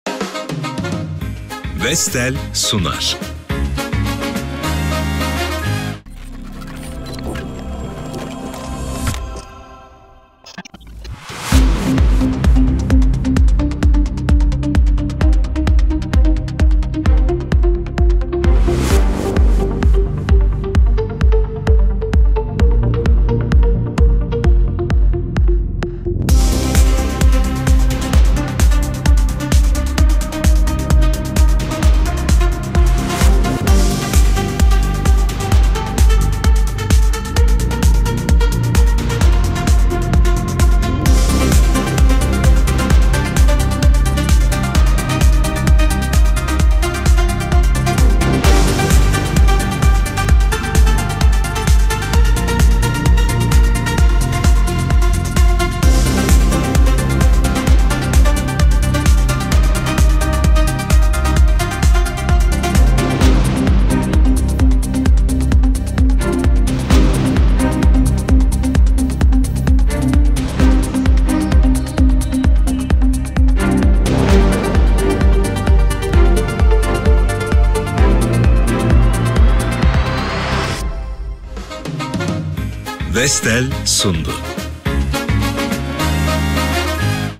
TeknOn ile teknolojide bu hafta (05-11 Temmuz)
VESTEL’in katkılarıyla hazırlanan Türkiye ve dünyada teknoloji alanında öne çıkan 10 gelişme.